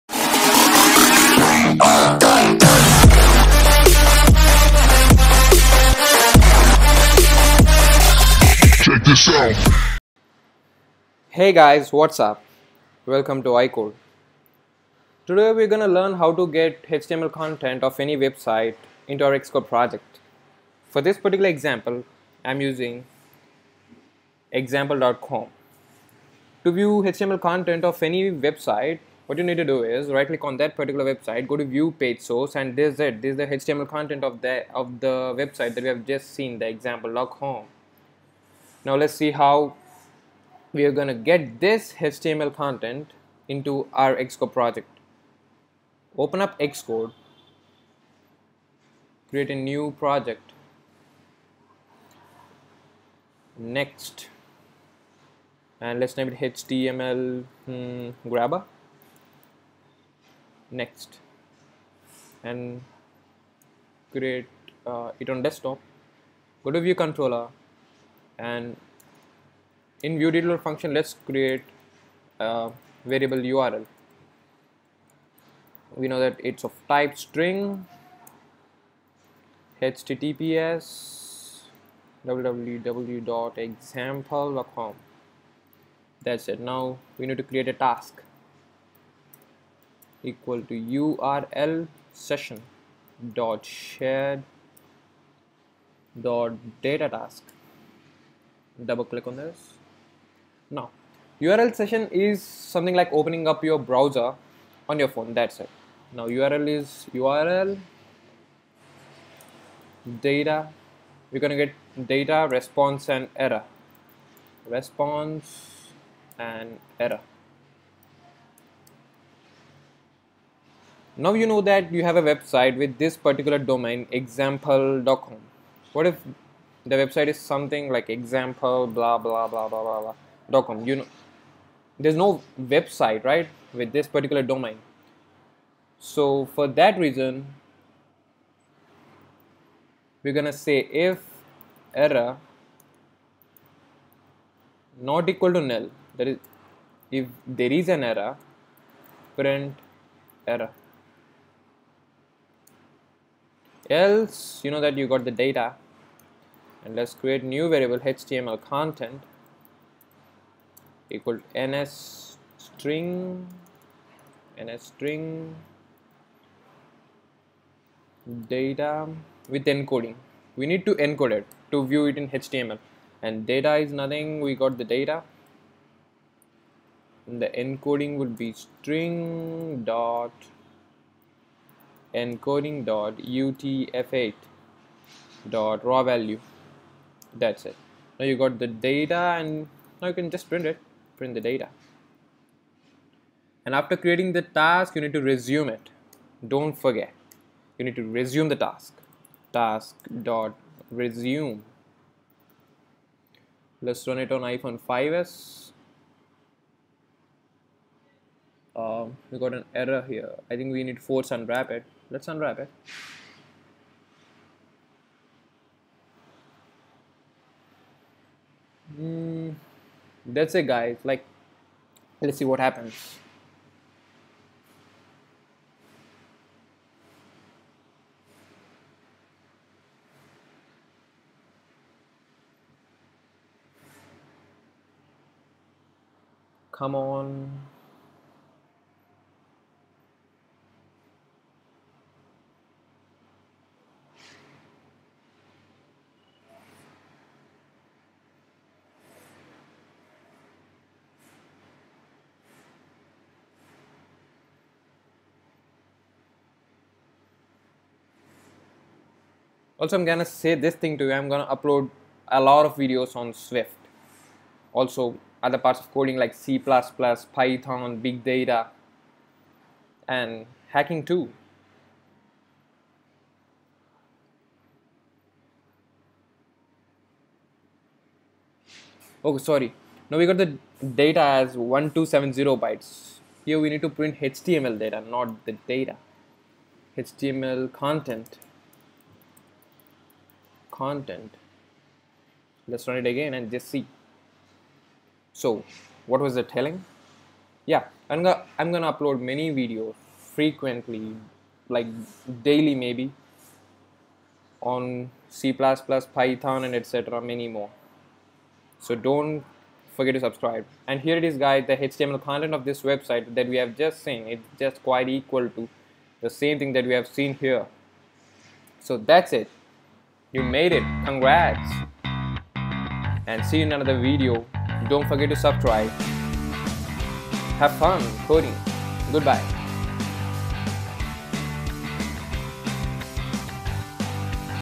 Getting HTML content into XCode in 5 minutes.(Swift 3)
This Tutorial helps you to get HTML content of any website into XCode